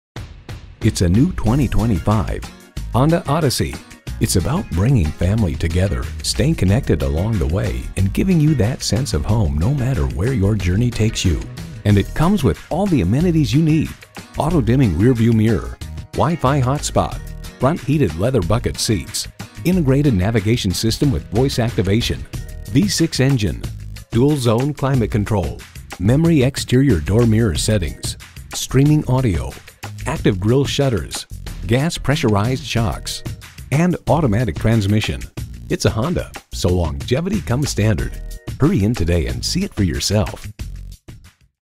New 2025 Honda Odyssey Washington DC MD Chantilly, DC HCSB009283 - SOLD
Year: 2025
Make: Honda
Model: Odyssey
Trim: Touring
Engine: 3.5L V-6 gasoline direct injection, i-VTEC variable valve control, regular unleaded, engine with cylinder deactivation and 280HP
Transmission: Automatic
Color: Solar Silver Metallic
Interior: Gray
Stock #: HCSB009283
VIN: 5FNRL6H84SB009283

Address:
4175 Stonecroft Boulevard